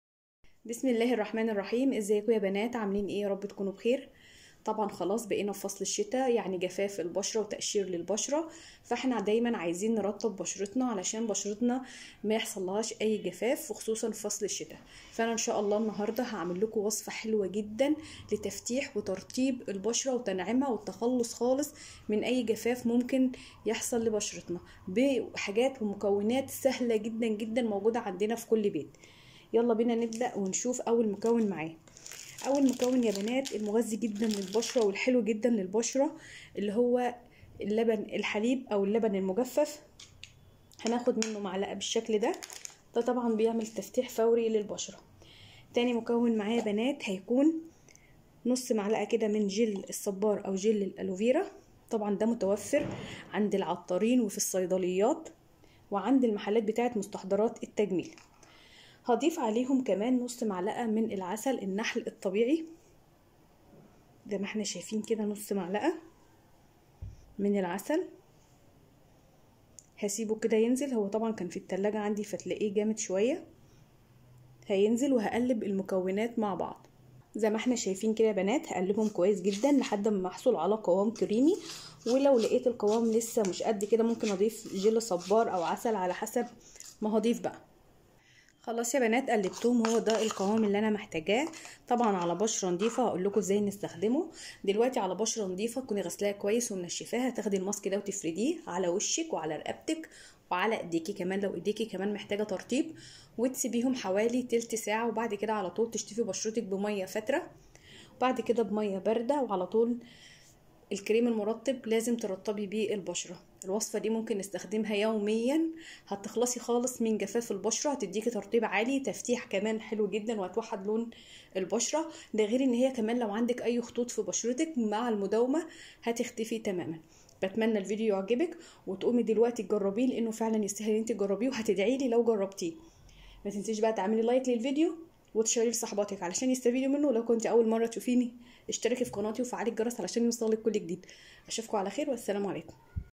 الماسك السحري للقضاء على جفاف وبهتان البشره والحصول على أفضل ترطيب وتنعيم ونضاره للبشره
أقوى وصفه لتنعيم وترطيب وتغذية و نضاره وصفاء البشره وتوحيد لونها